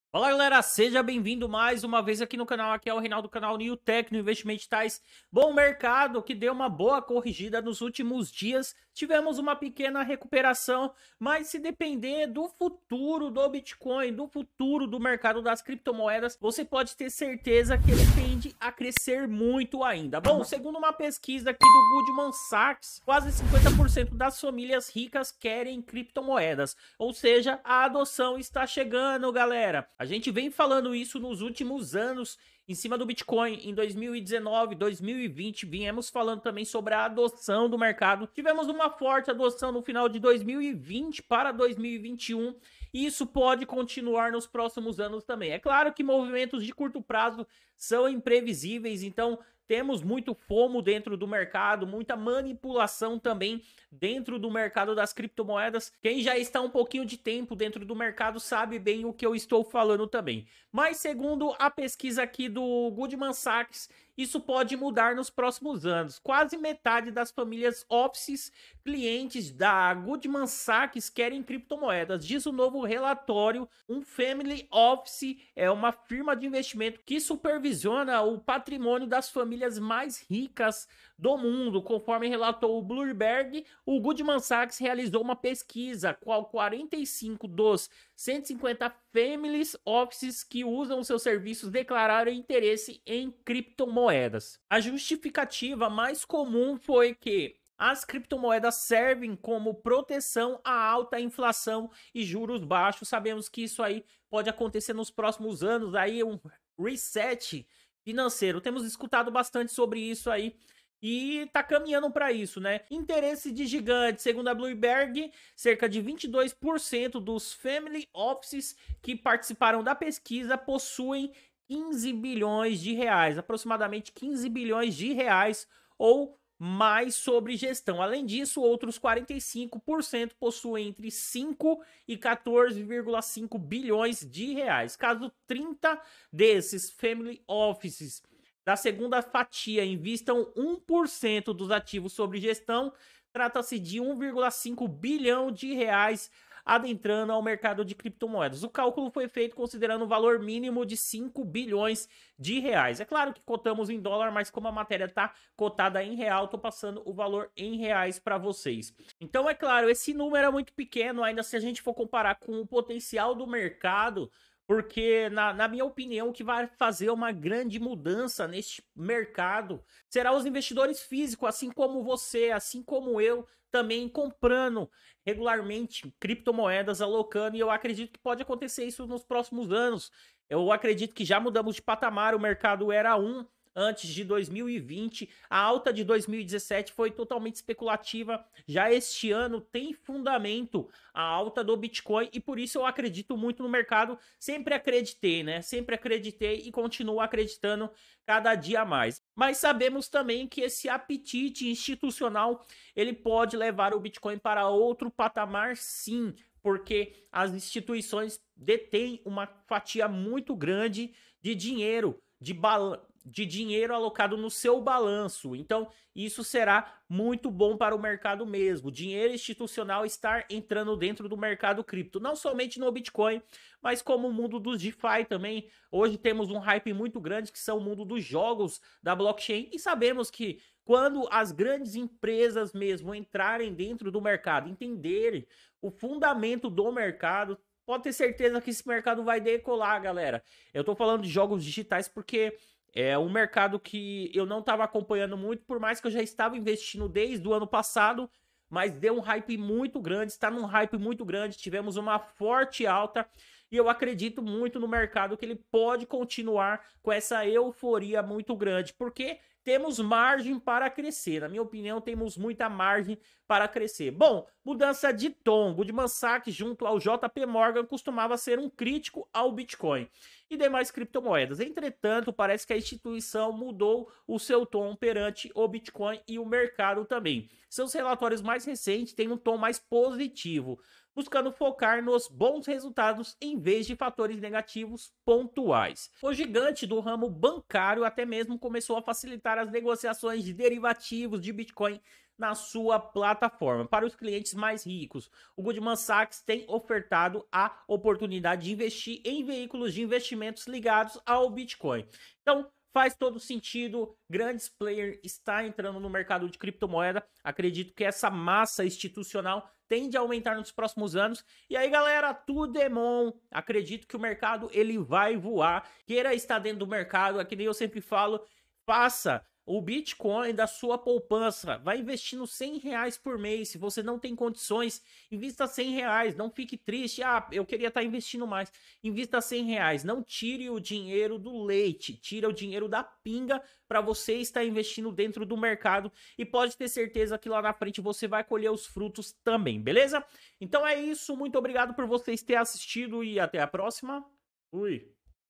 Quase metade dos clientes do Goldman Sachs querem investir em criptomoedas !!!
✅Seja membro deste canal e ajude o nosso canal !!!

CANAL DO TELEGRAM PARA CONTEÚDOS EXCLUSIVOS !!!